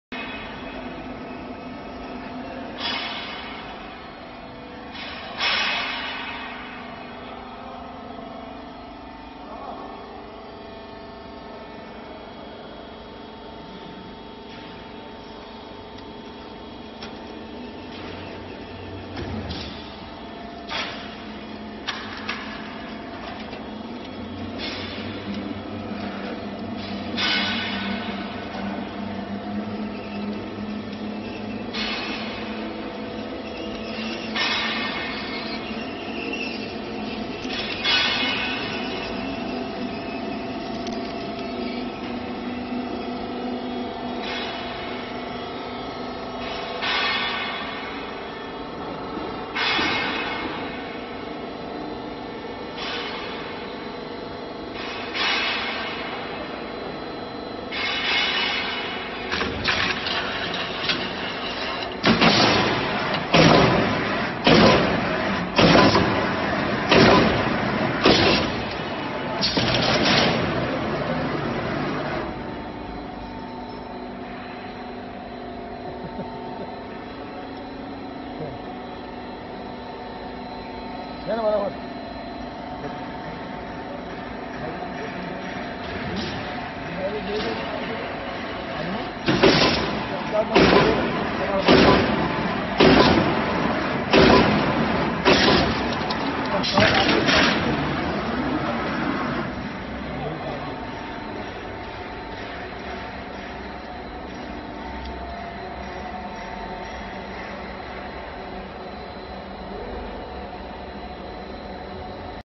High speed roller shutter door forming machine
Servo tracking shearing type roller shutter door forming machine
High speed production
Perfect product